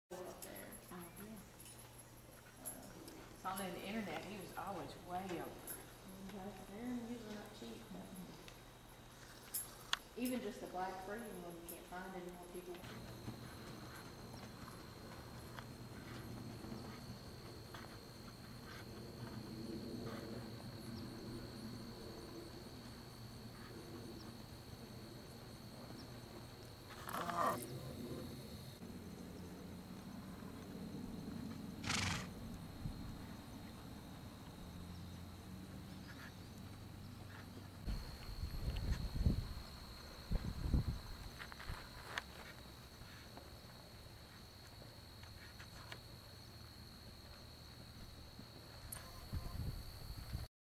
TWH - Mr. Edge
Registered TWH gelding. Measures 16.2 hands, smooth gaited, field trial or trail riders dream mount!